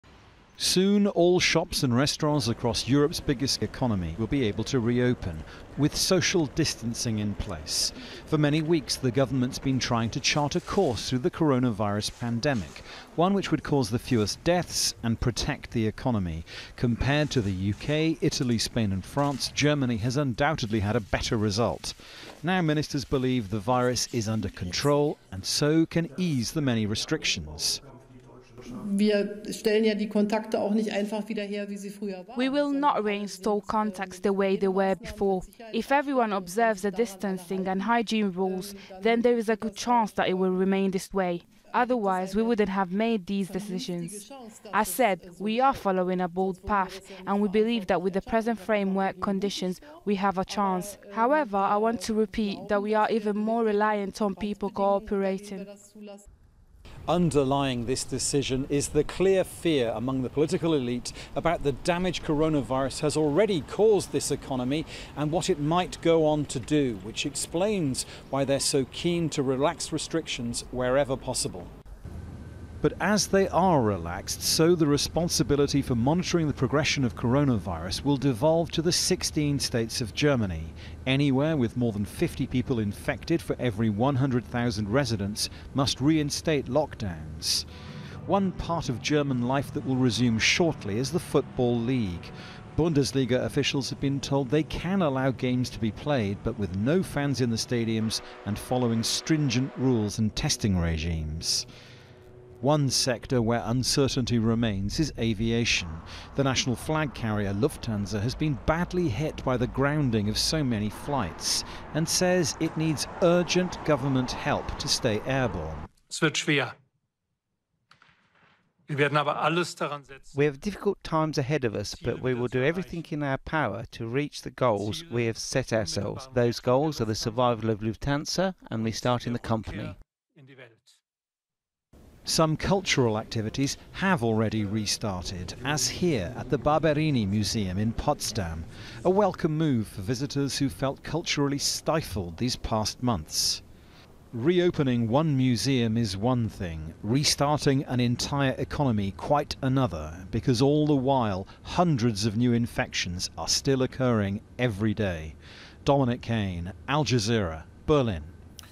Germany lockdown: Gradual reopening as virus cases taper off
Germany is easing its lockdown after weeks of restrictions put in place to stem the spread of the novel coronavirus.
More shops and museums will reopen and top level football will return to the pitch.
The government hopes these moves will gradually help Europe's biggest economy recover.

Al Jazeera's Dominic Kane reports from Berlin, Germany.